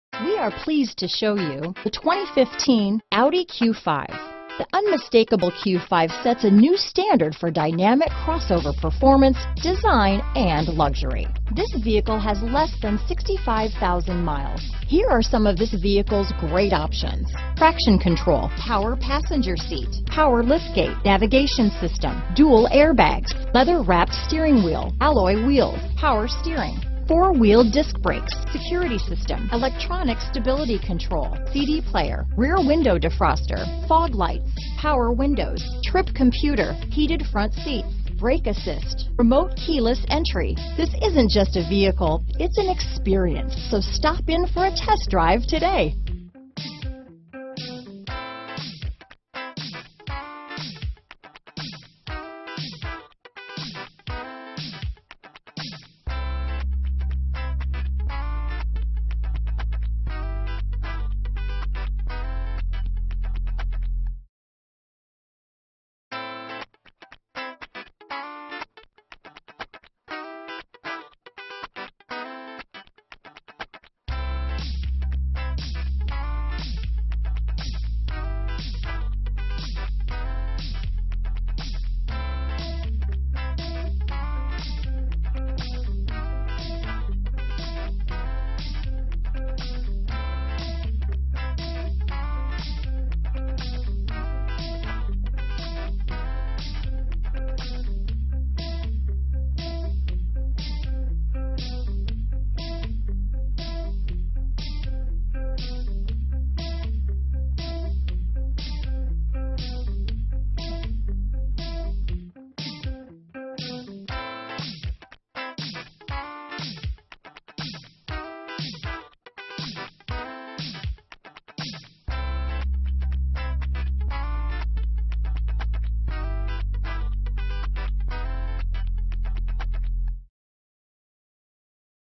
2015 Audi Q5 West Palm Beach, Palm Beach Gardens, Royal Palm Beach, Wellington, Near me PLM019365
Brilliant Black Used 2015 Audi Q5 available  in West Palm Beach, Florida at Napletons Palm Beach Acura. Servicing the Palm Beach Gardens, Royal Palm Beach, Wellington, FL area.
2015 Audi Q5 2.0T Premium Plus - Stock#: PLM019365 - VIN#: WA1LFAFP8FA019365